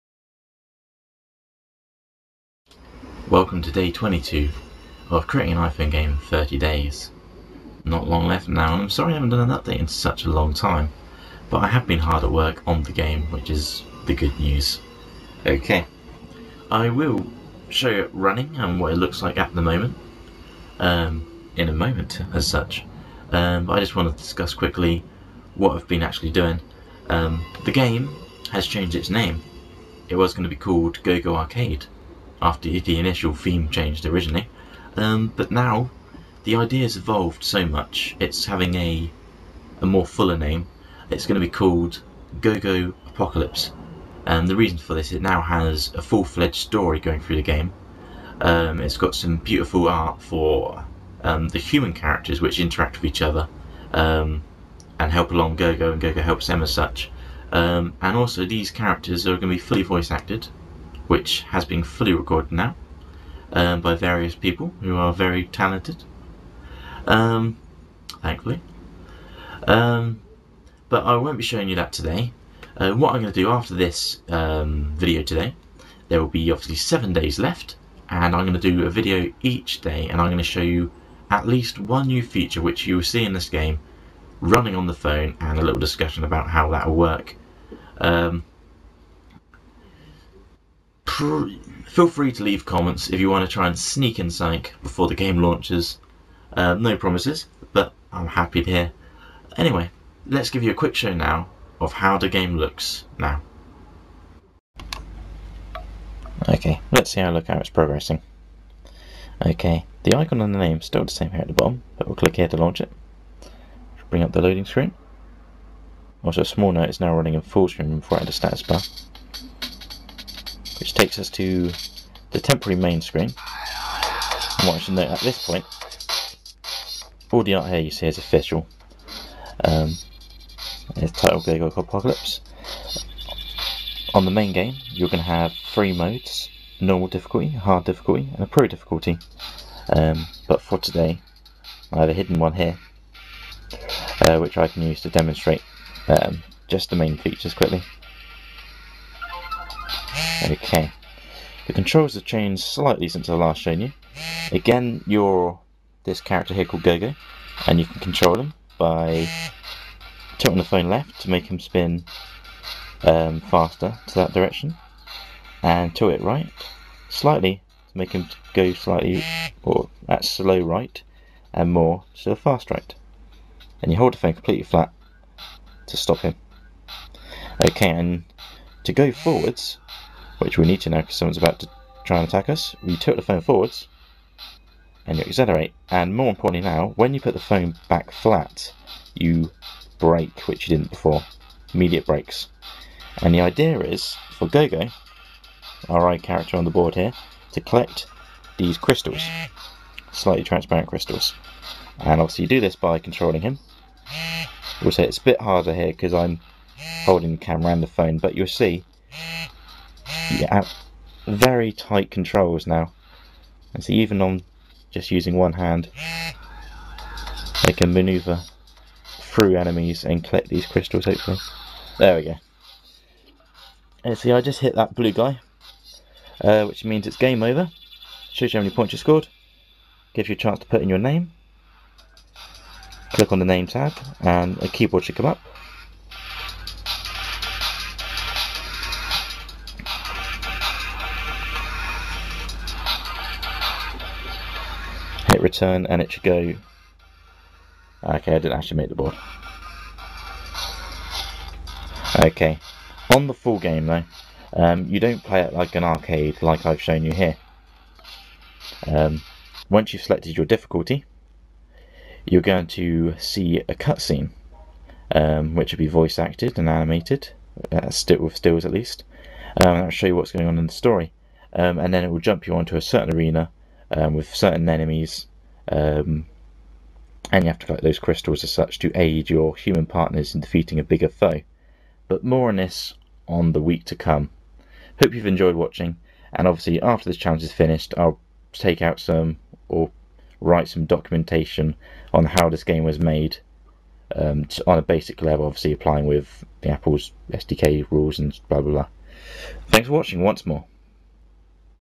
Day 22, the 30 day make an iphone game challenge
Here is day 22 of the 30 Day challenge to make an iPhone game, sorry its taken so long! but lots of good news!, the game is coming on really well today im going to show you alittle of this and talk you through the controls and the main aim, from here on out expect a new video every day till the BIG 30!